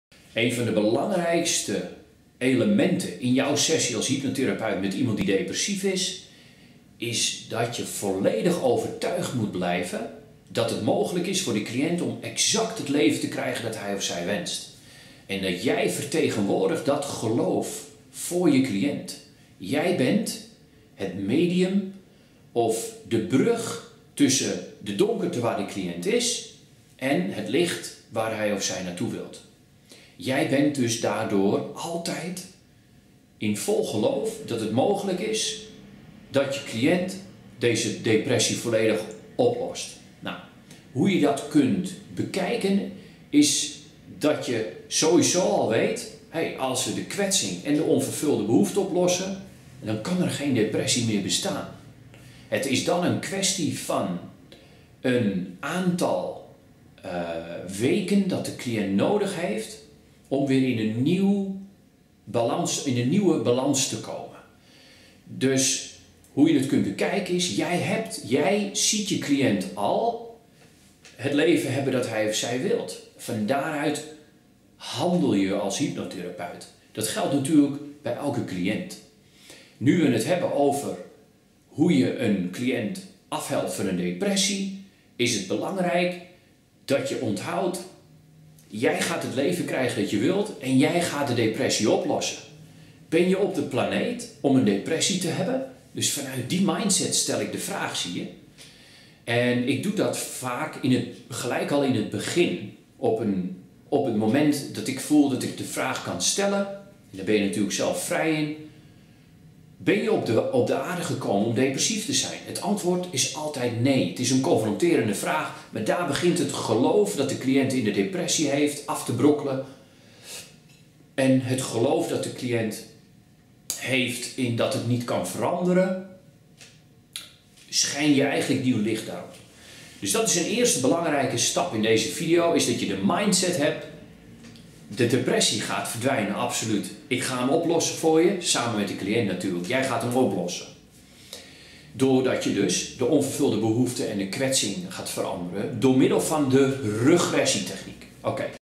Depressie oplossen (D2 De mindset)- Martijn Groenendal- Hypnose -coaching protocol -
Dit protocol is onderdeel van het Complete Hypnotherapie-Opleidingspakket